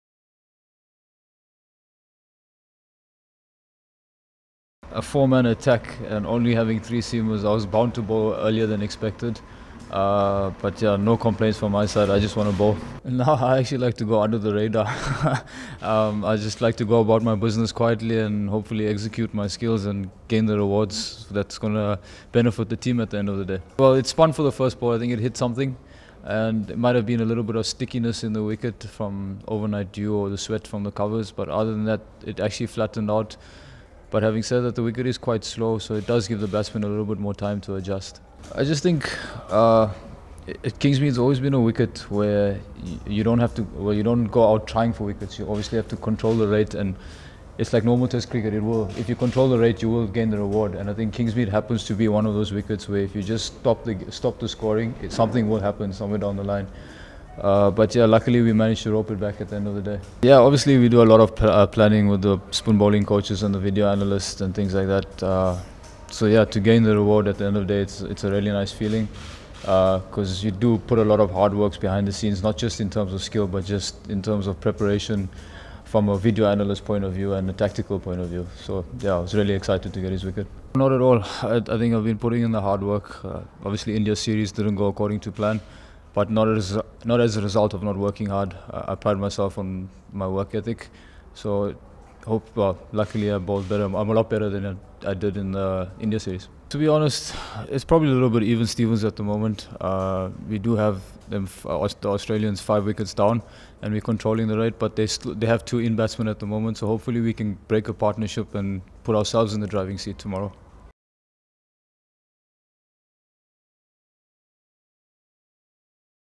Maharaj claims vital wickets on Kingsmead debut
KESHAV MAHARAJ celebrated his home ground Test match debut with the key wickets of Steve Smith and Shaun Marsh on the opening day of the Sunfoil Test Series between the Standard Bank Proteas and Australia at Sahara Stadium Kingsmead on Thursday.
When bad light brought an early close with Australia on 225/5 after 76 overs after the visitors had won the toss on a good, although slow, batting surface, the match was evenly poised.
There could have been little doubt that the Proteas would have preferred to get a full day in fading light as they were only four overs away from the second new ball and only one wicket away from breaking through into the Australia lower-order. Maharaj was key to the Proteas strategy after the Proteas decided to strengthen their batting with the inclusion of Theunis de Bruyn and field only four specialist bowlers. The strategy was sound as the surface was one that allowed Faf du Plessis to employ him early in the day and he sent down the most overs of any of the Proteas attack (2/69 in 24 overs). His role and that of Vernon Philander, a typically economical and penetrative 2/36 in 17 overs, meant that the Australians were never able to get away to the free-scoring start they normally wish to employ. There were two 56-run partnerships – the third wicket between David Warner (51 off 79 balls, 6 fours) and Smith (56 off 114 balls, 11 fours) and the fourth wicket between Smith and Shaun Marsh (40 off 96 balls, 6 fours). For the rest the Proteas kept chipping away at regular intervals until Mitchell Marsh and Tim Paine added an unbroken 48 for the sixth wicket, leading to the close.